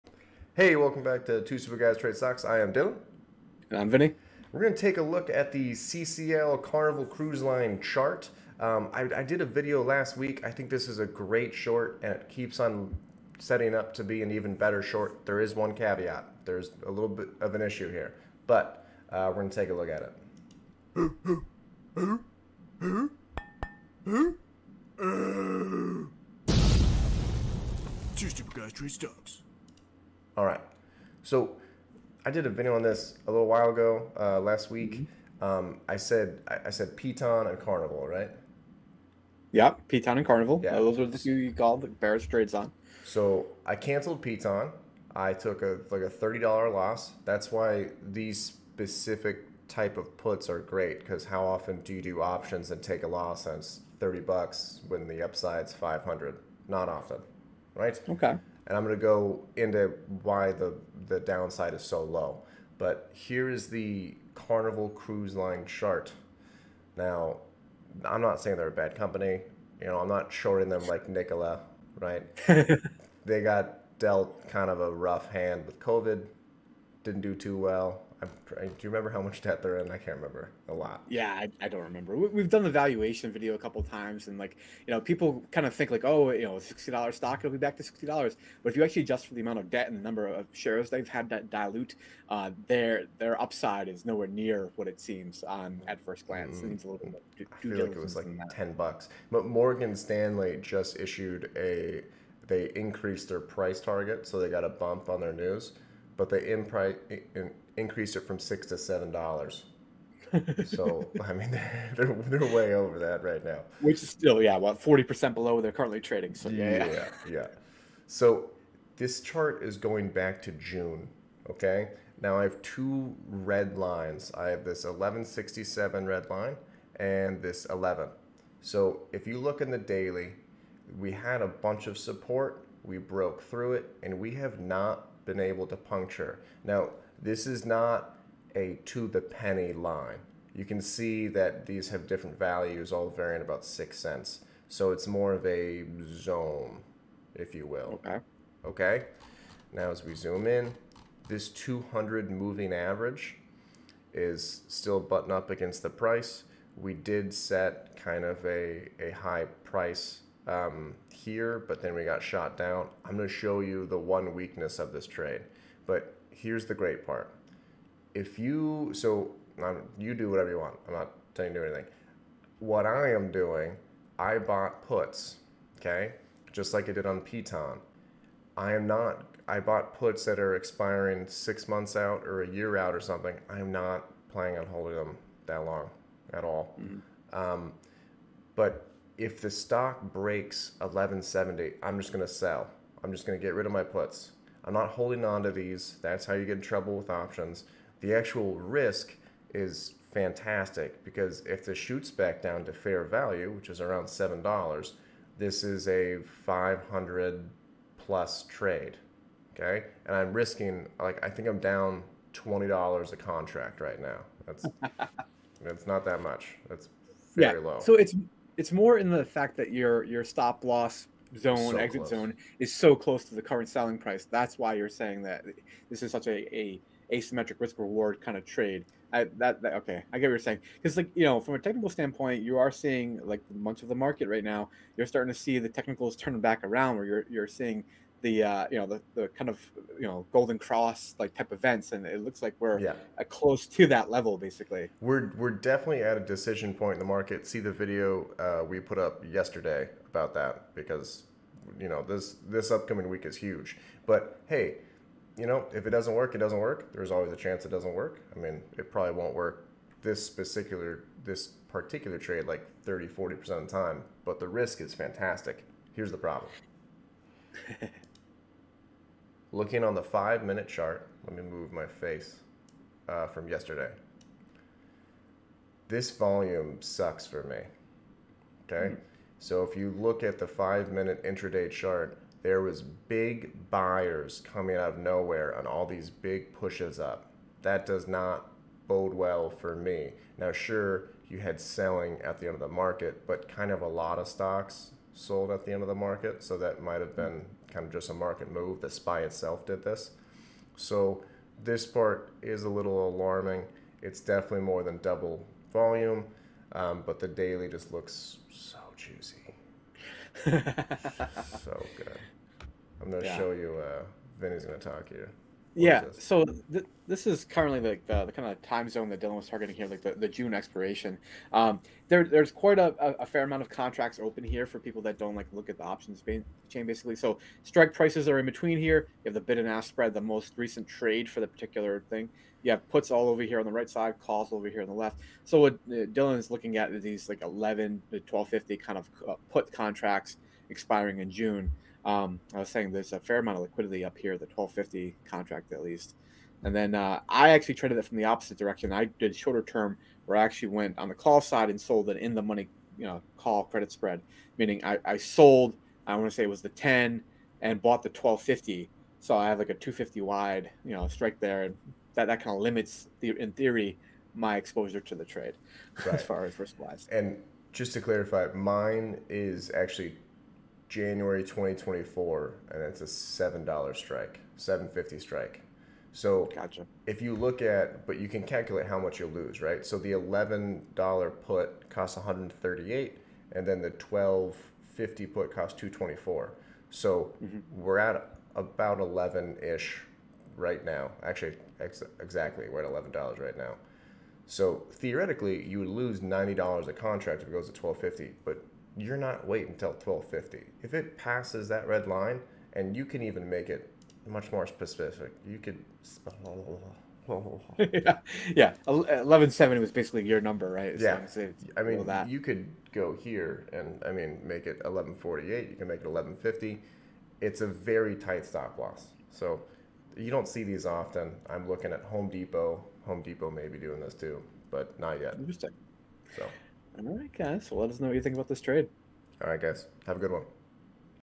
Swing Trade of the Month; Short option on Carnival Cruise Line
I think this may be the best swing trade Short option on the market right now. I love the trades where you may lose 50 but can win 500. You never know which its gonna be but the risk to reward is amazing. Obviously take at your own risk!

Dylan Robinhood referral
Join Robinhood with my link and we'll both get a free stock 🤝